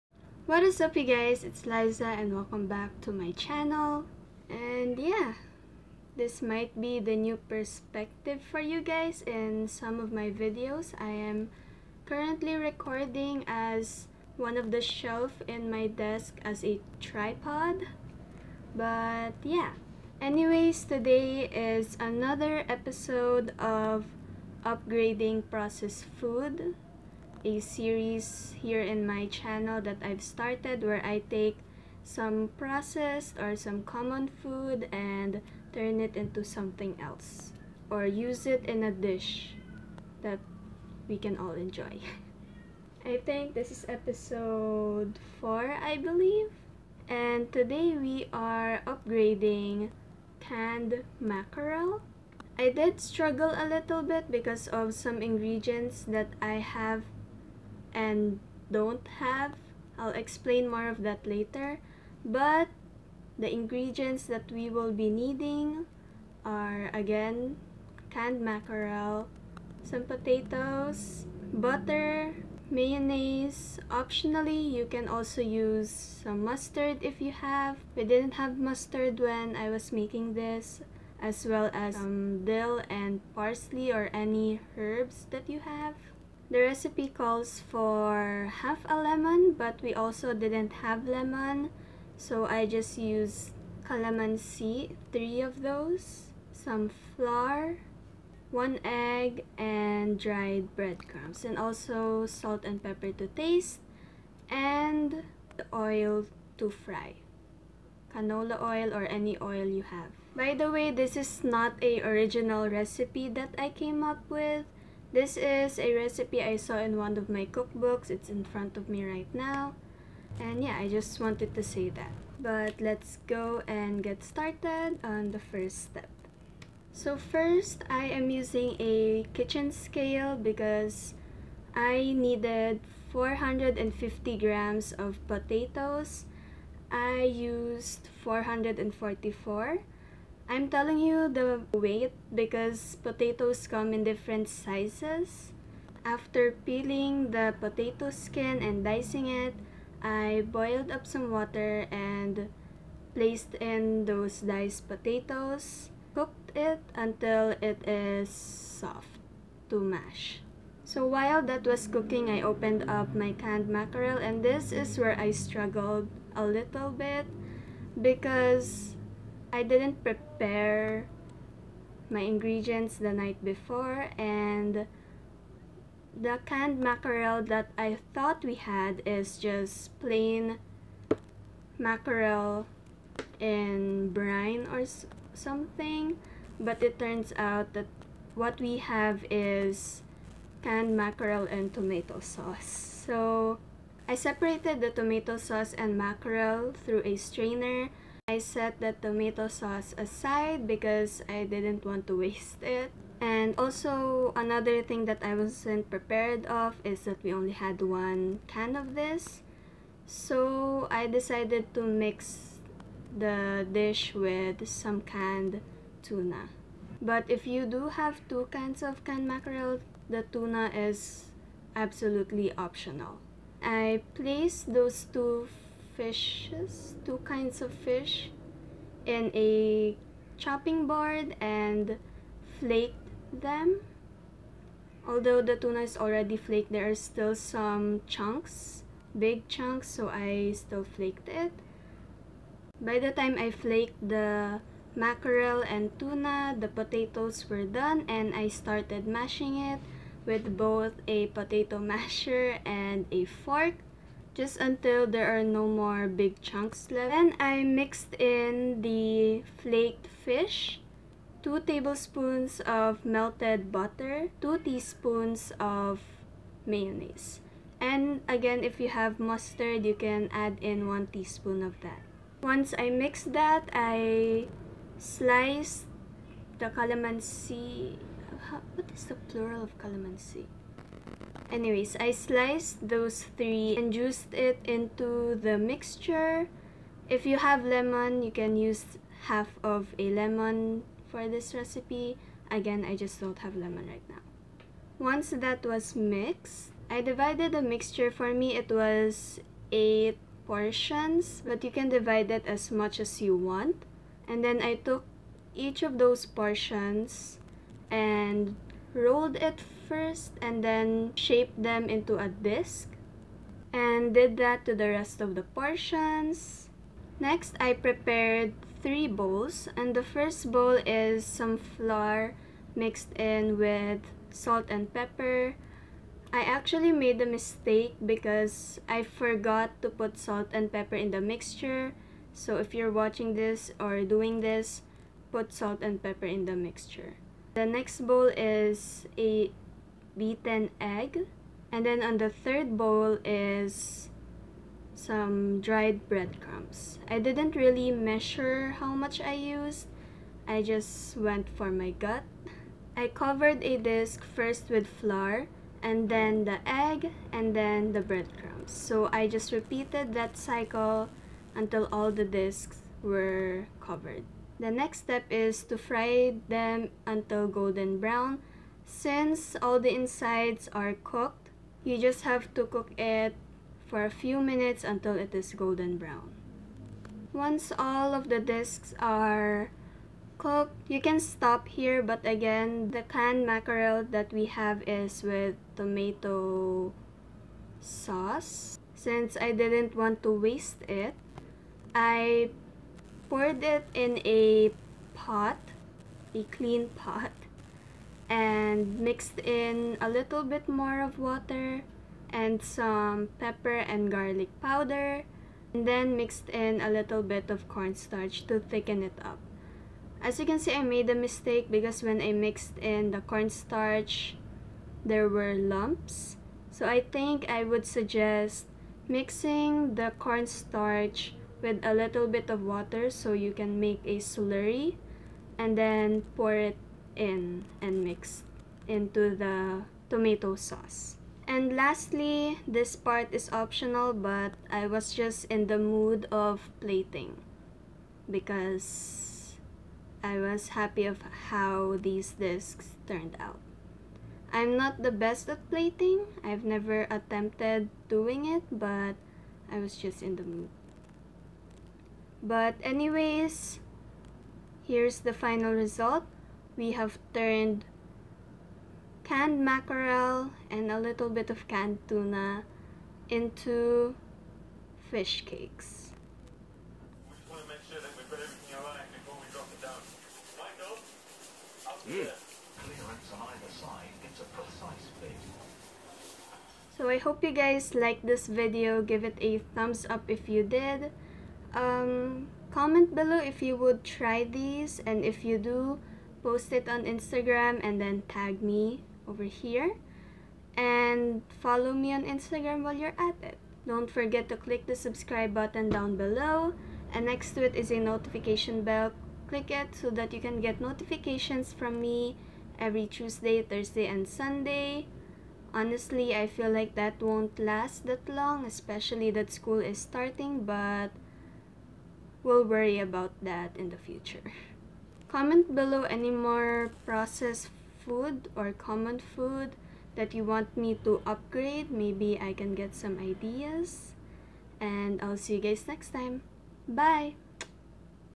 UPGRADING PROCESSED FOOD #4: Canned Mackarel
HEY GUYS!!! Welcome to another episode of UPGRADING PROCESSED FOOD, and today, we are upgrading CANNED MACKAREL with a little bit of canned tuna.